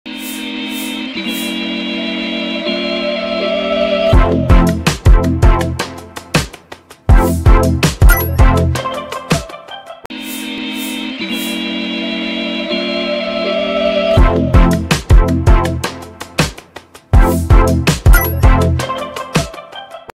Maya with Free InstaPay Transfers and Scan to Pay Digital Ad Q3 2023 10s x2 (Extended Promo) [HD/ST]
Enjoy FREE InstaPay transfers to other banks and e-wallets with Maya.

DRPS One 9890: Maya with Free InstaPay Transfers and Scan to Pay Digital Ad Q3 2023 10s x2 (Extended Promo) [HD/ST]